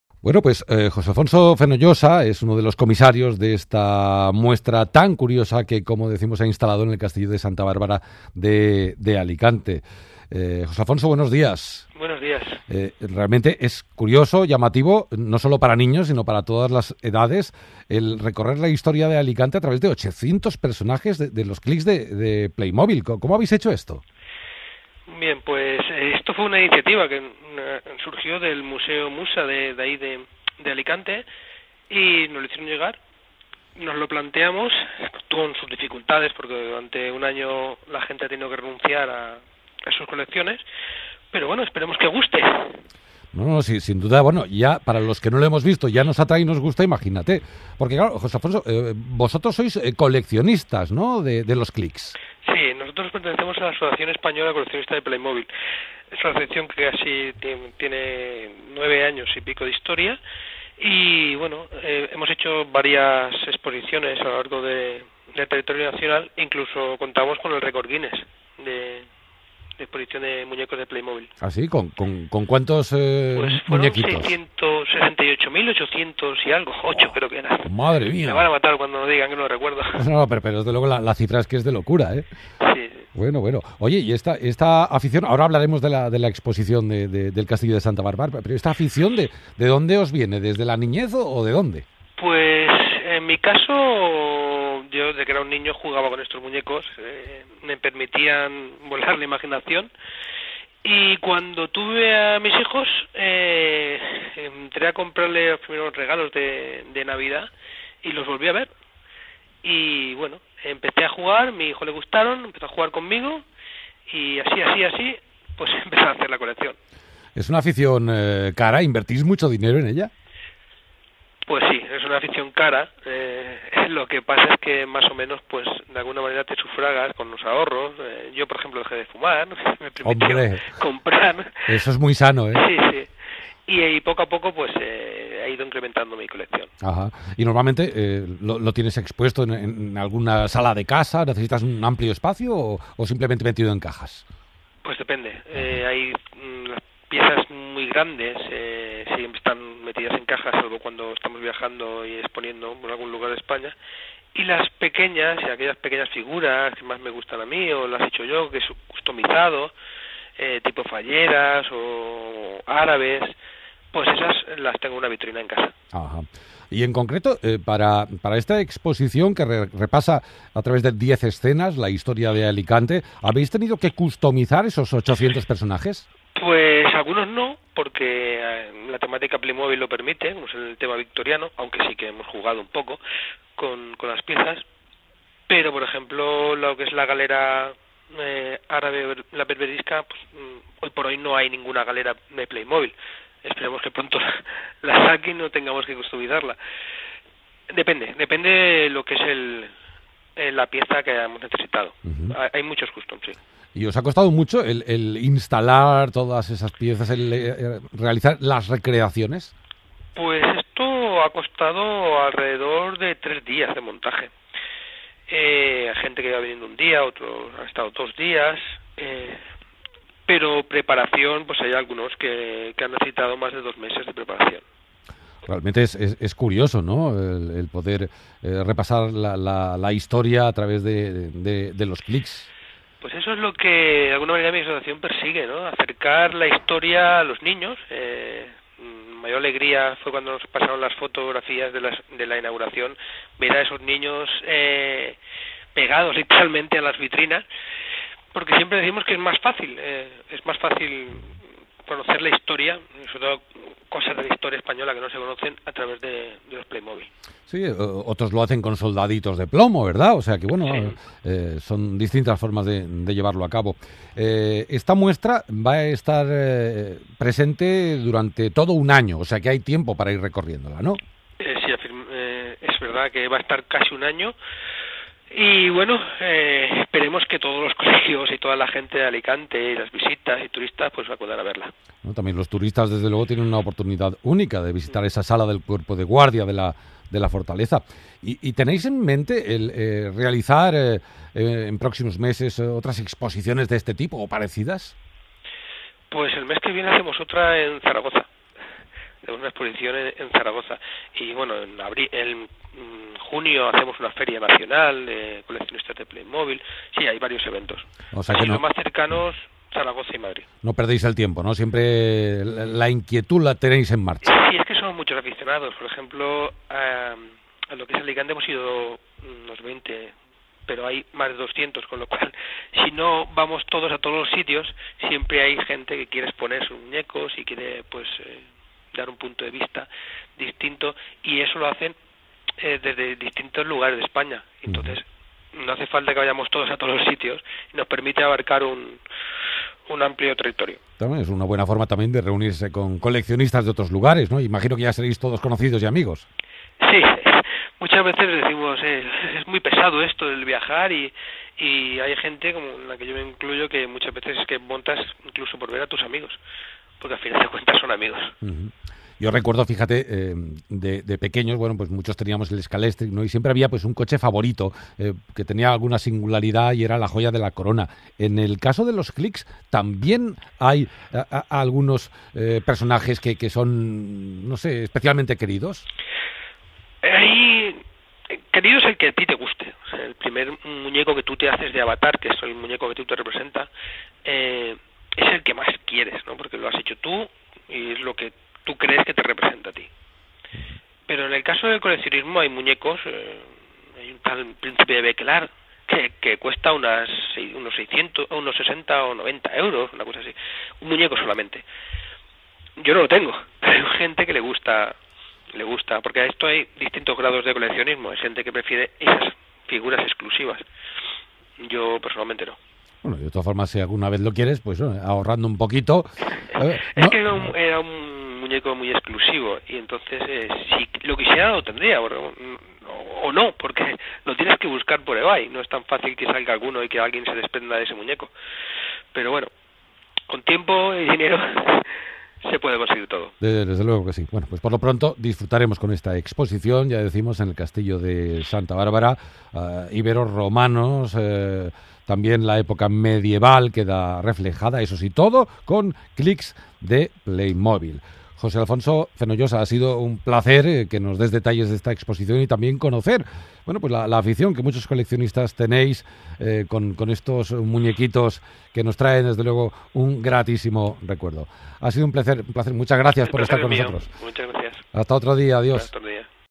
Entrevista en Cadena Ser Valencia (Expo Alicante)
Con motivo de la exposición "Los Clicks toman el Castillo" que Aesclick tendrá en el Castillo de Santa Bárbara, Alicante hasta marzo del 2014 en Cadena Ser Valencia le hicieron una entrevista a Halcon28, uno de nuestros socios y comisario de la exposición.